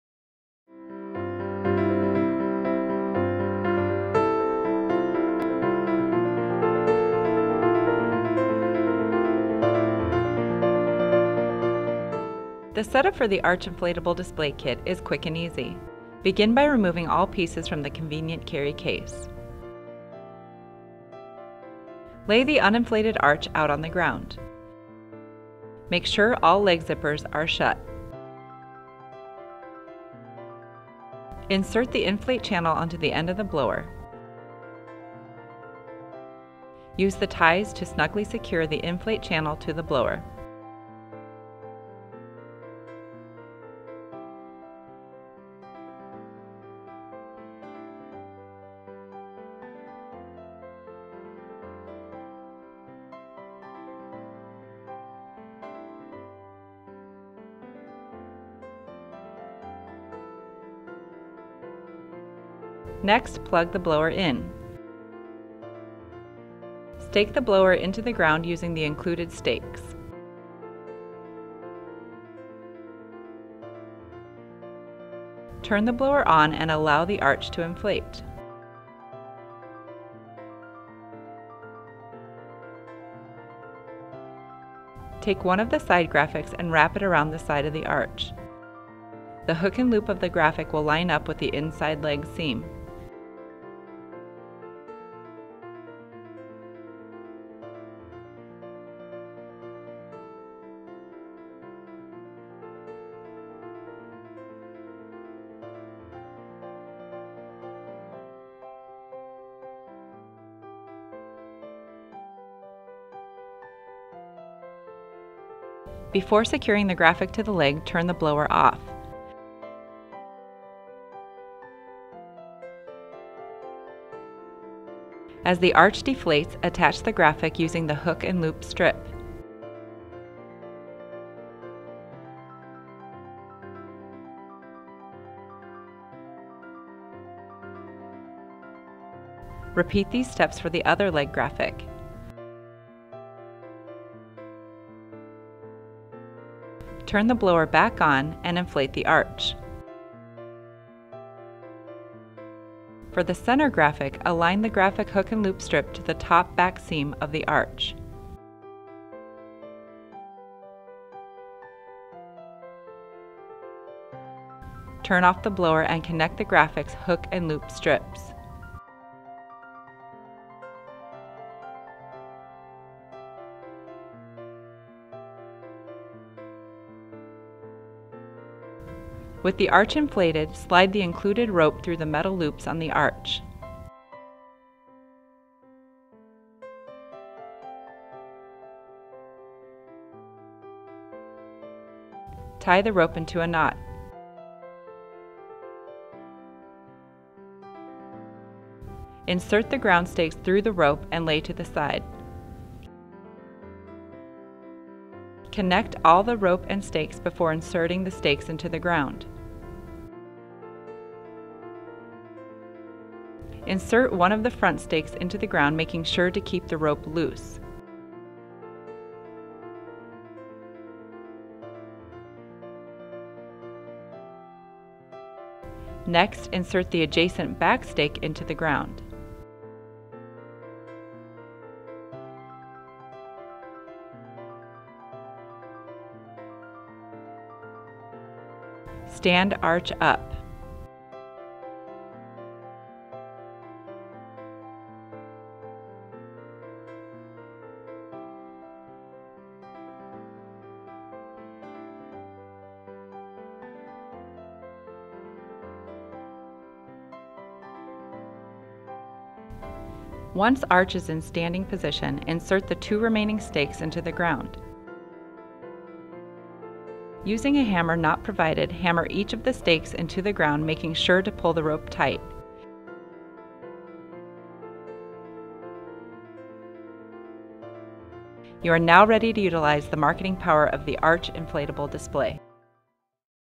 Arch Inflatable Display - GLM Displays
Create a grand entrance with inflatable archways.
• PVC-coated nylon material is lightweight, water-resistant and tear-resistant
• Graphics are dye sublimated on 400 denier polyester
• Inflates in under a minute